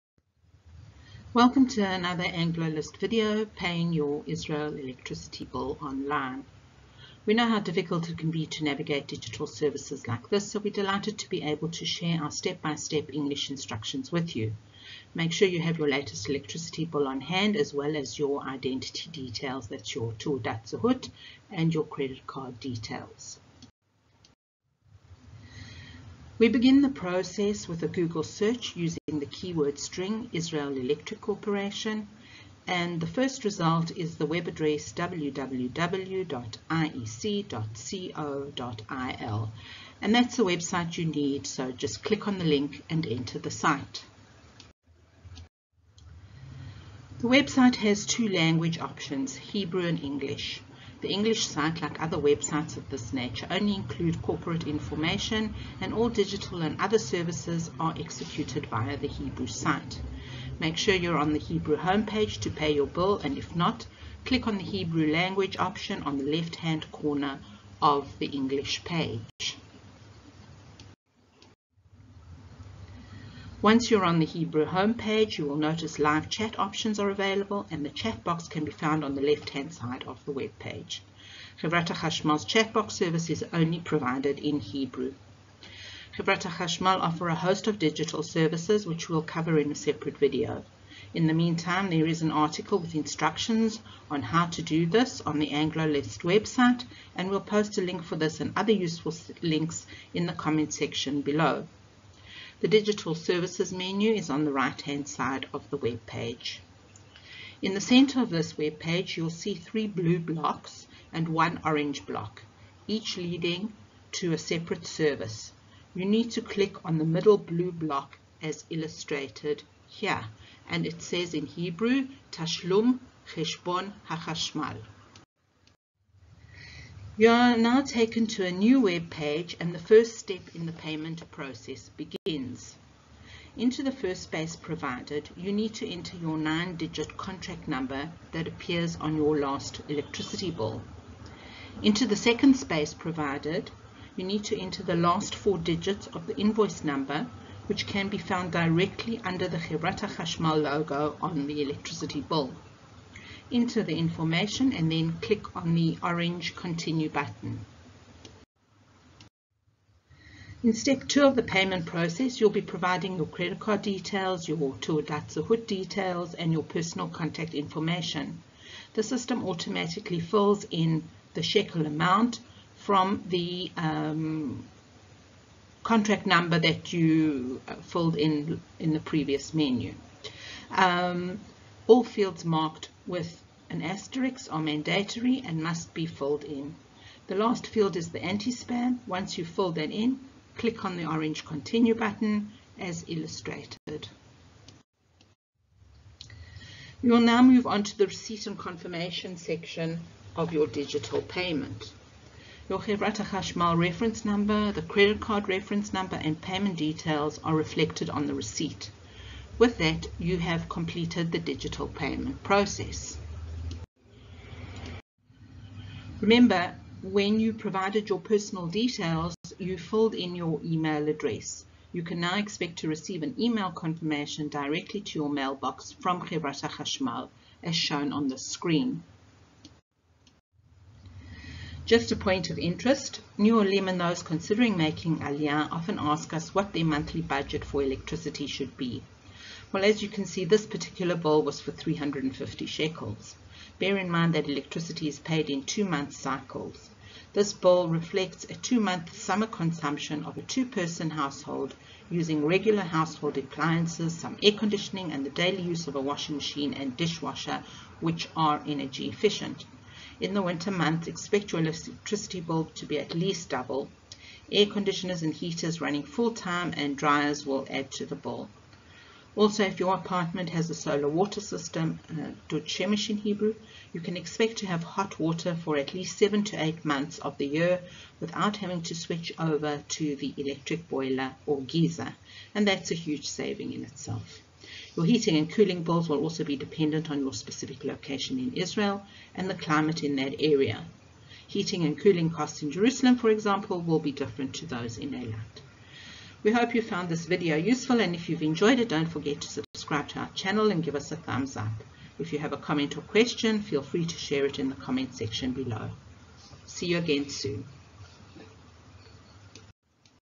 English instructions: Paying your Israel Electricity Bill from Hevrat Hahashmal (IEC) Online
A step-by-step instructional video showing you how to pay the Israel Electric Corporation household electricity bill online.
*Extra resources like electricity rates, transferring the bill to your name, electricity rates, discounts and saving tips can be found